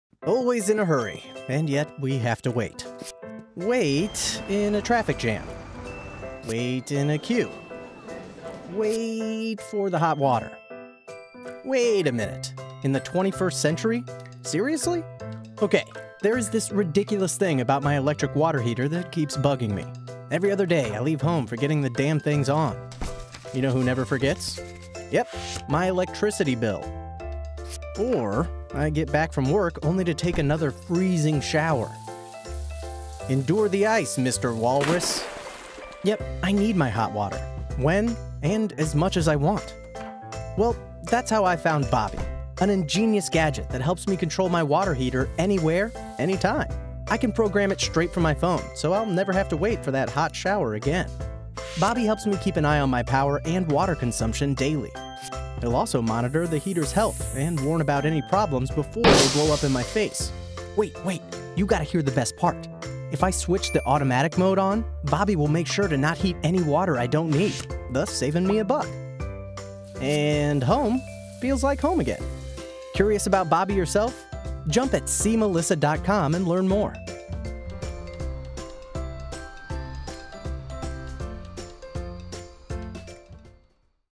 Bobbie - The Smart Water Heater Controller
Мееt Bobbie - The smart water heater controller
Have hot water whenever you need it and control your water heater from your smartphone.